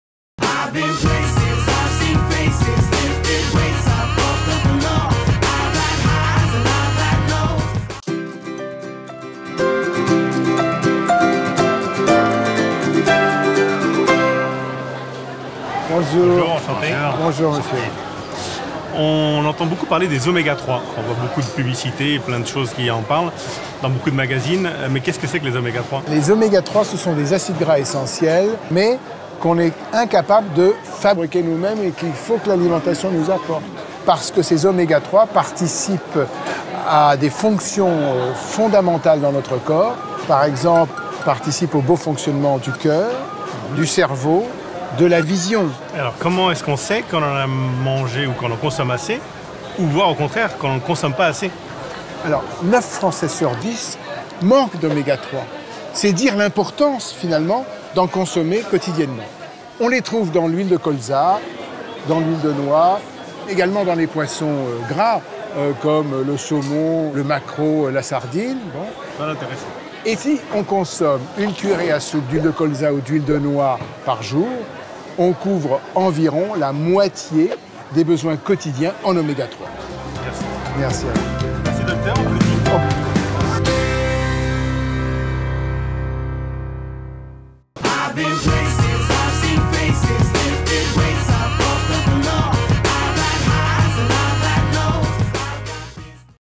Comment rendre digestes et appétissantes des informations nutritionnelles ?
Et si on parlait Nutrition ? pour Lesieur, avec le Dr Patrick Serog